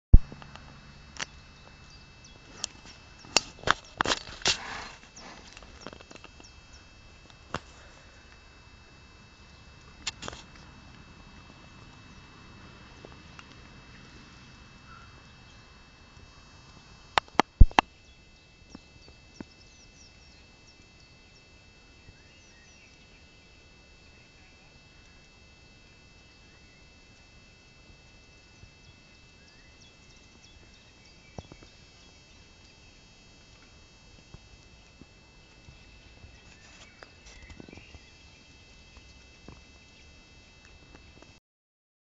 hest der gaber - horse who yawns
Min søde hest en sommerdag - og som gaber